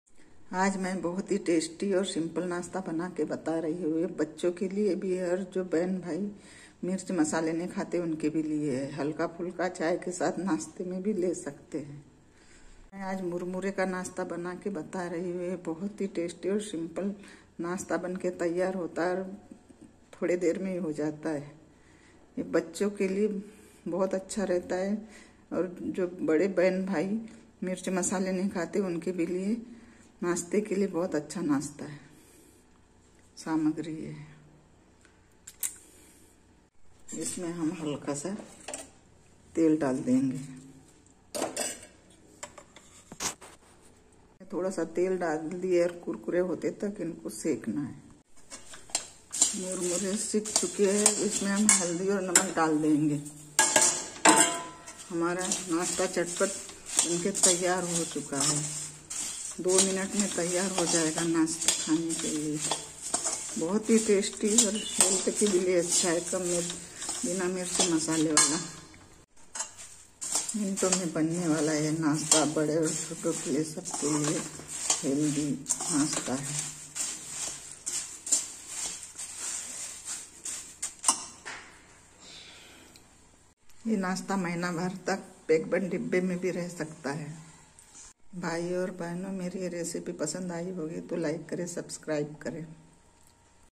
Cooking food: मुरमुरे का स्वादिष्ट नास्ता बनाये हैं मिनटों में (बच्चों और बड़ों की पसंद)
सामग्री: 250 ग्राम मुरमुरे स्वादानुसार नमक एक चोथाई 1 छोटी चम्मच तेल चम्मच हल्दी